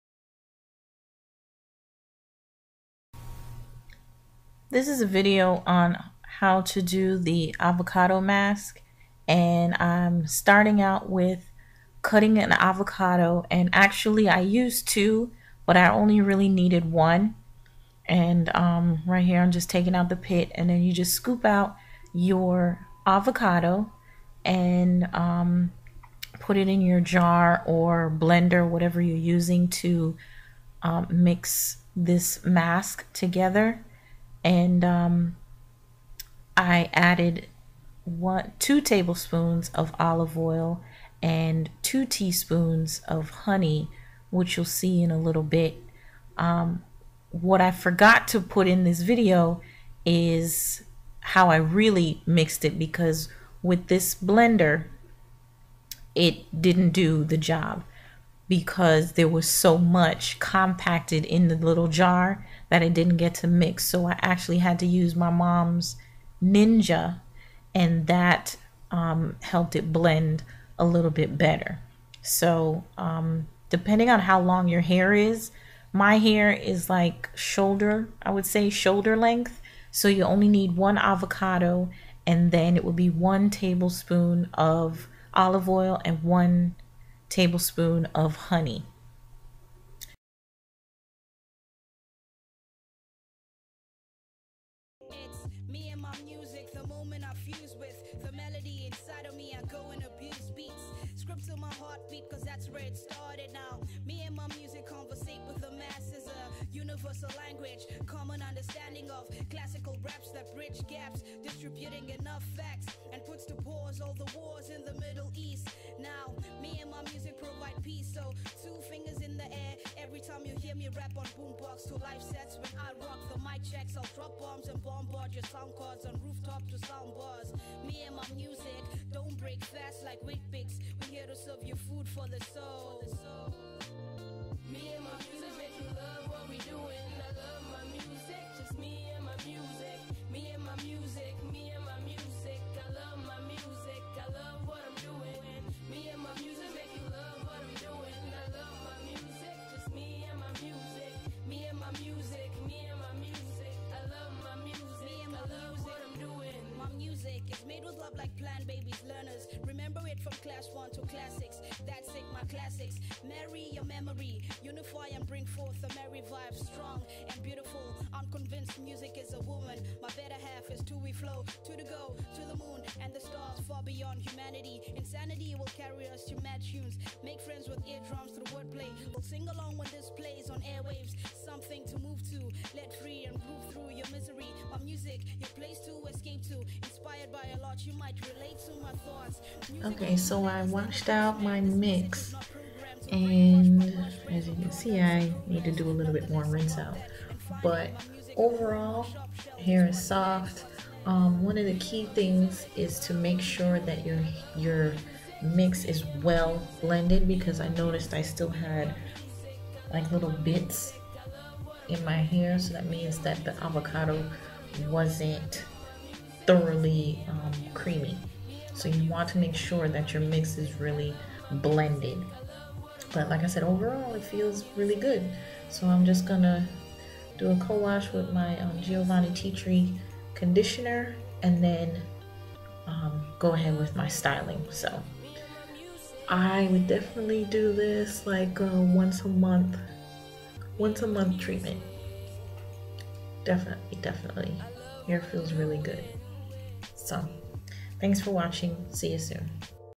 DIY Deep Conditioner Avocado Mask |Natural Hair
After watching a video on 20pearlsNcurls channel about avocado mask, I had to try it on my natural hair.  I used the following to make the mask:

2 avocados / 2tbsp olive oil / 2tbsp honey